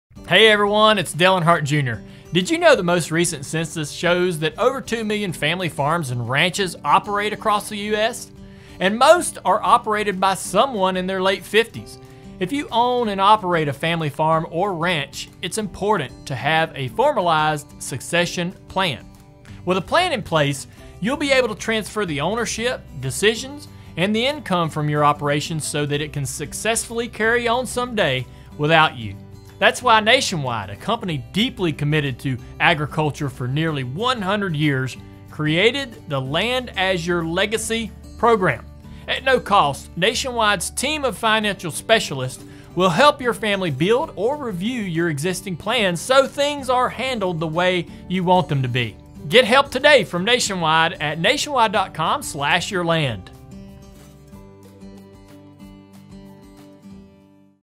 Nationwide Land As Your Legacy – Preserving Your Family Farm or Ranch
Farming and ranching families are aging and too few have a formal plan to transition their operation to the next generation.  Nationwide’s Land As Your Legacy® team of transition planning specialists can help.